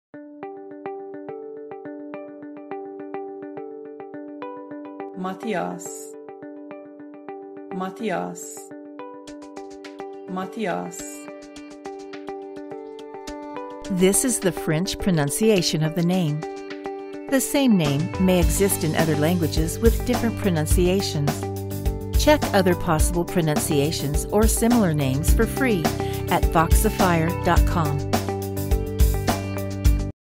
How to Pronunce Matias in French - Voxifier.com
This is the French pronunciation, gender, and phonetics recording of the name Matias. The same name may exist in other languages with different pronunciations.